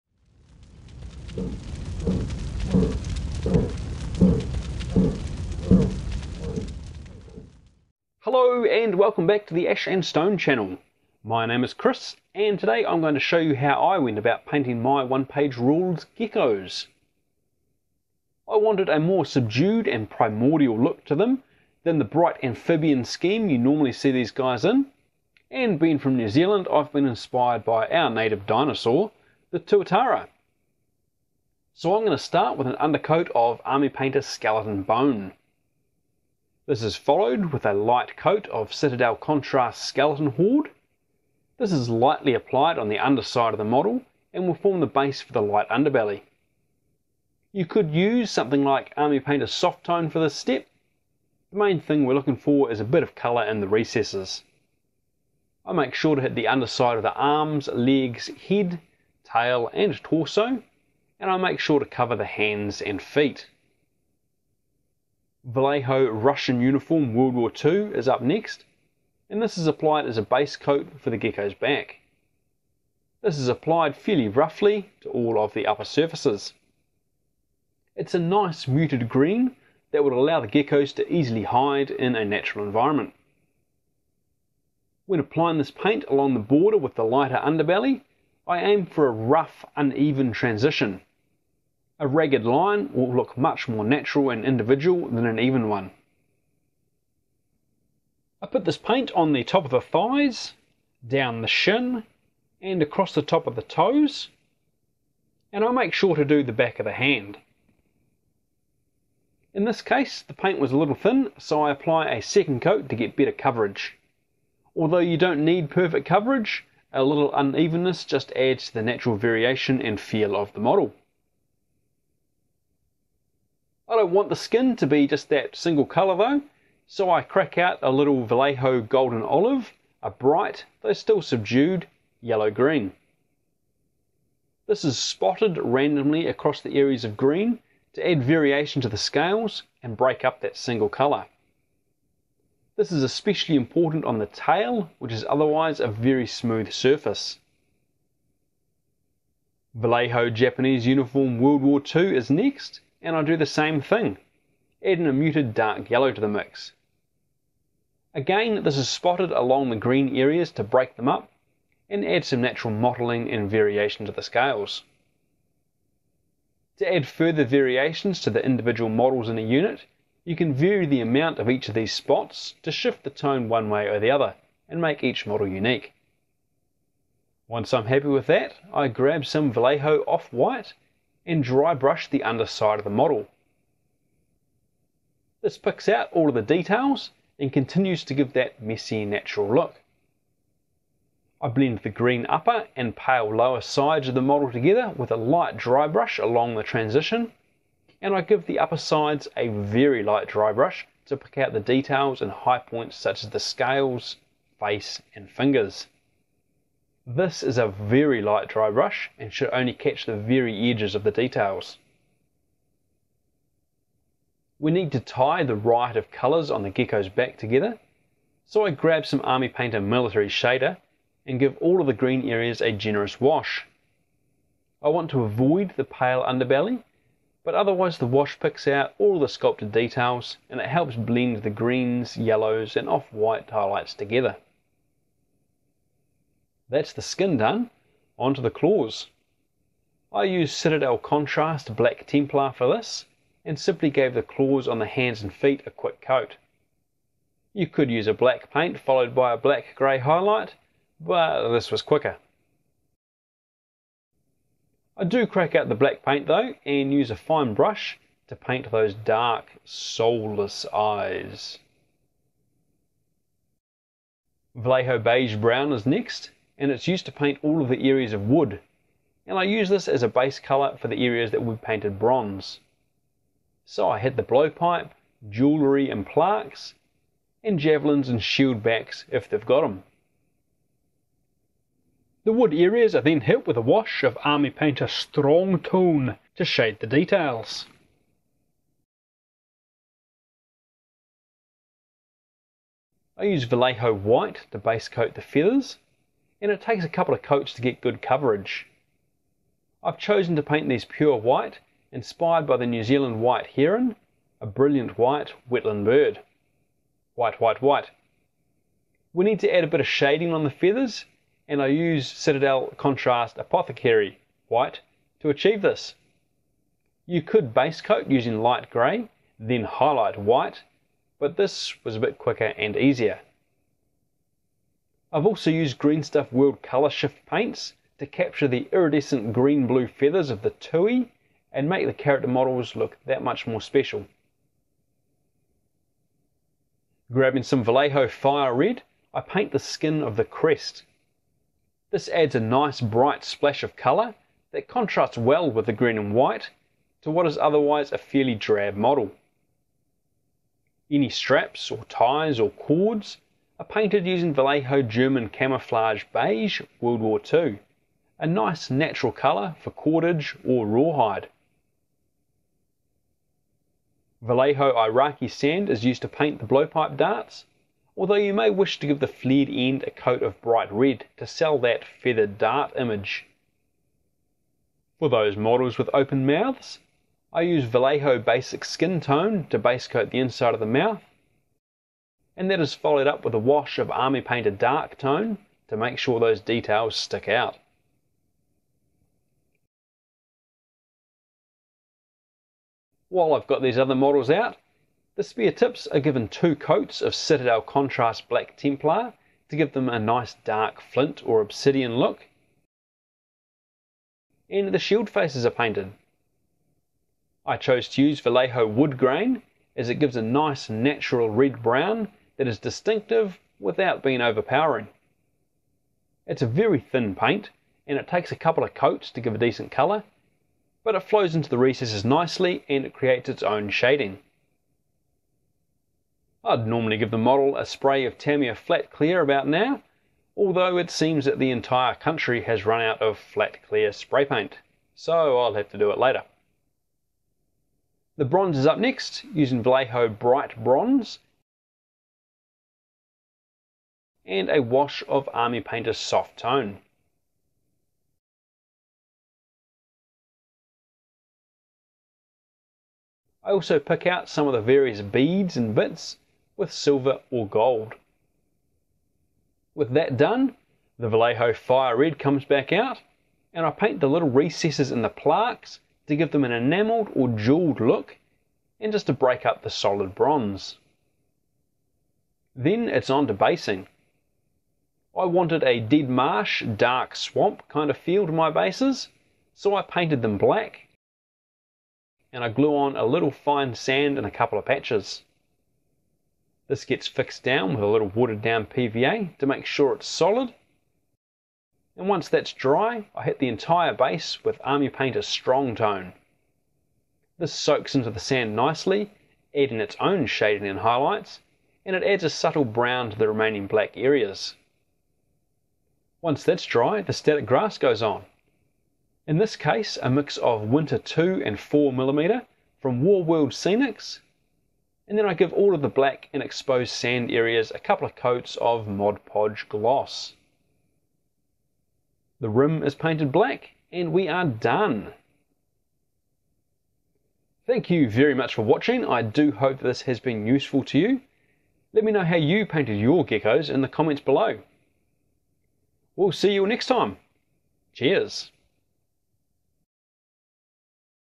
Painting One Page Rules Geckos
A quick guide on how I'm painting my OPR Geckos as primordial swamp dwellers.